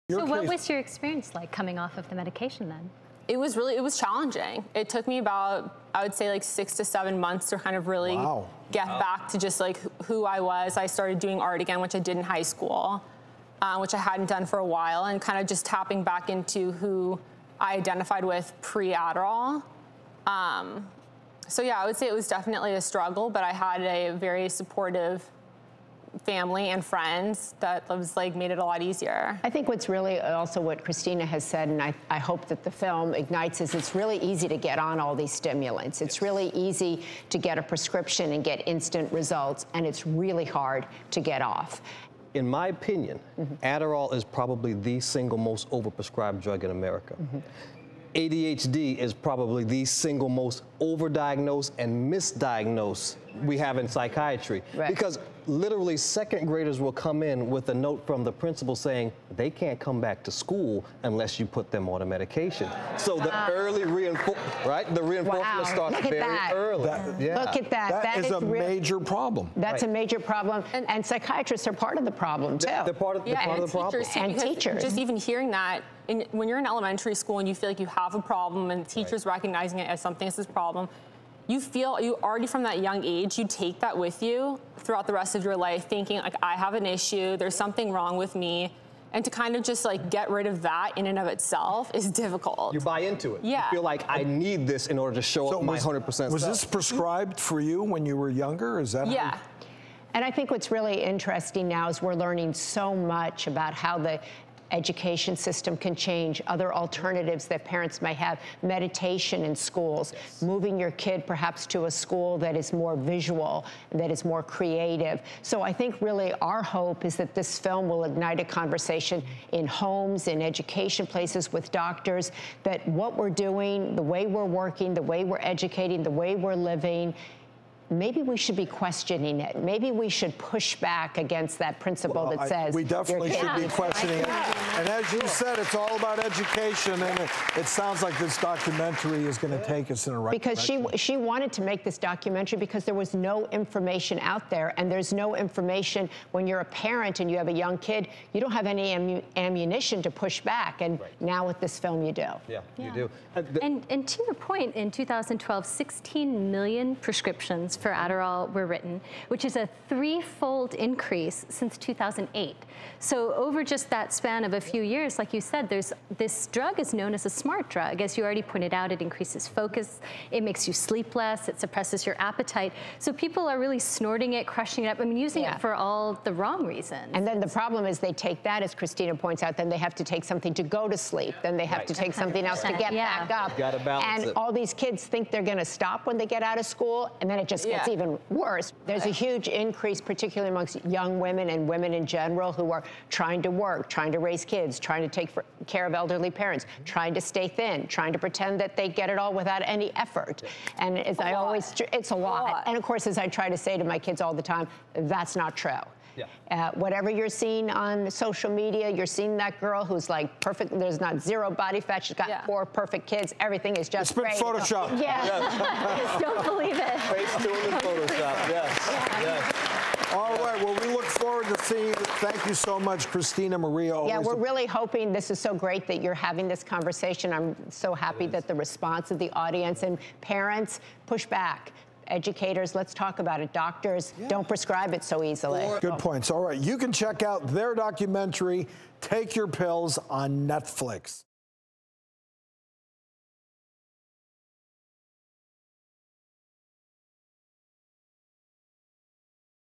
Christina Schwarzenegger Reveals How She Quit Using Adderall
Maria Shriver and Christina Schwarzenegger, executive producers of the documentary “Take Your Pills,” discuss the drastic increase in Adderall use among children and college students. Plus, Christina reveals how she was able to quit.